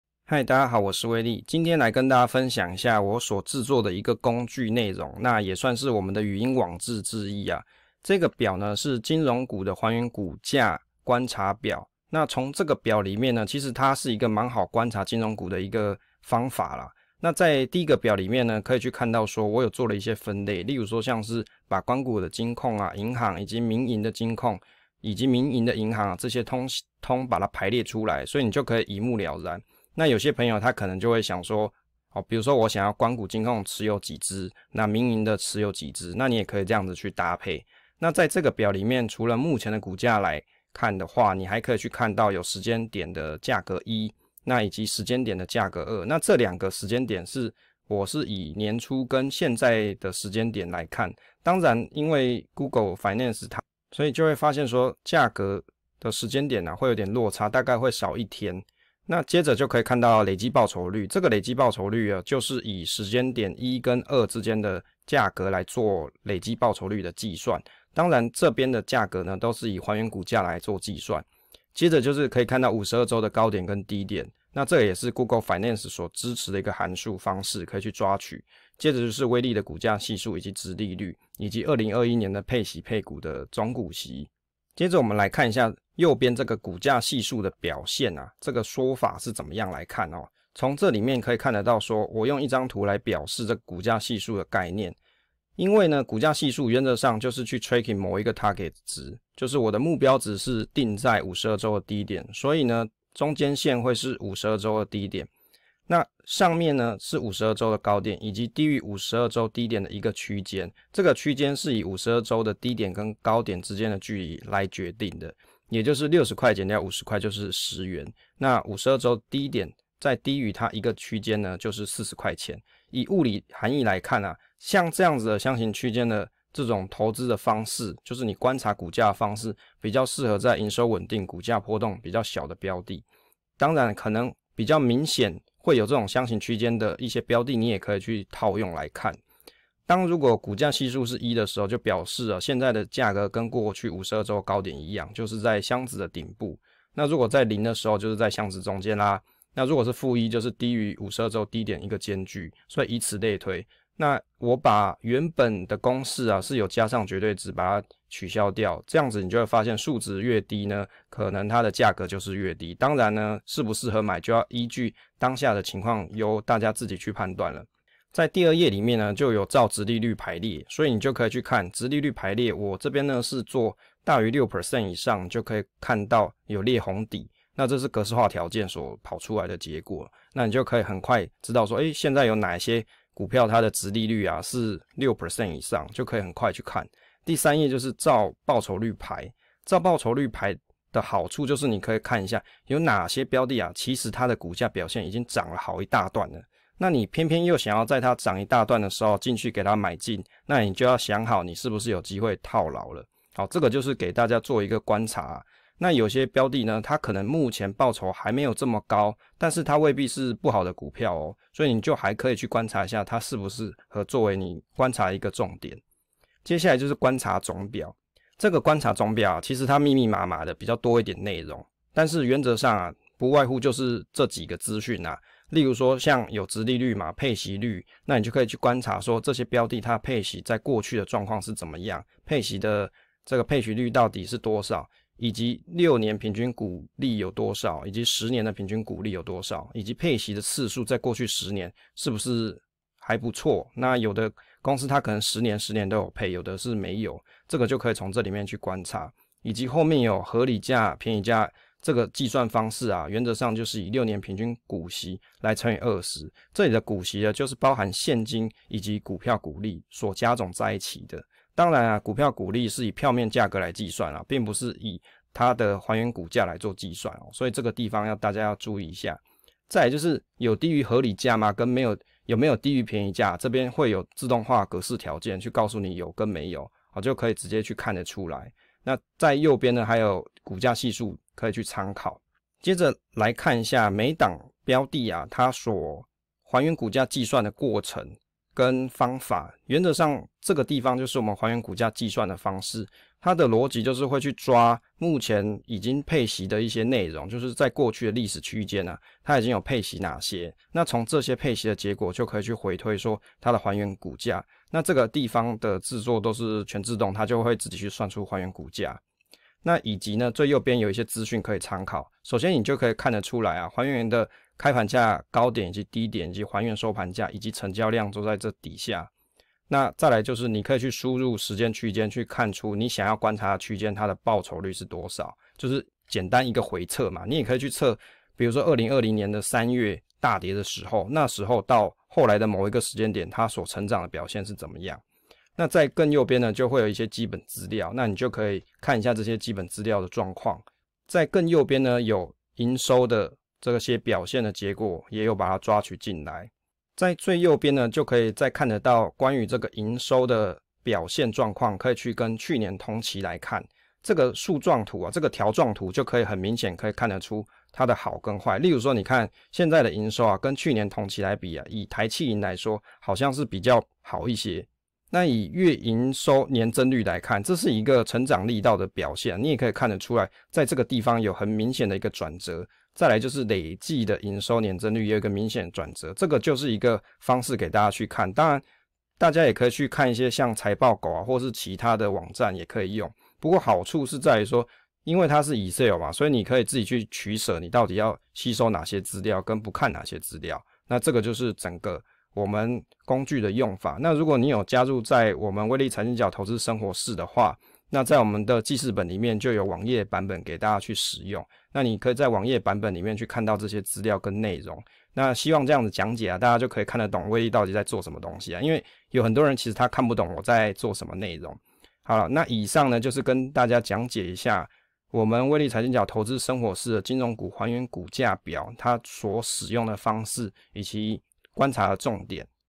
金融股還原股價績效觀察表
用這個影片跟大家分享如何使用TOOL做觀察，有講話

2022威利財經角金融股觀察工具 報酬率採用還原股價計算、自動抓股息、抓報價、計算52週係數、還原股價曲線繪製，給喜愛金融股的朋友觀察使用。網頁連結放在[威利財經角投資生活事 ] 社群，群貼11，已加入的讀友可以自行使用。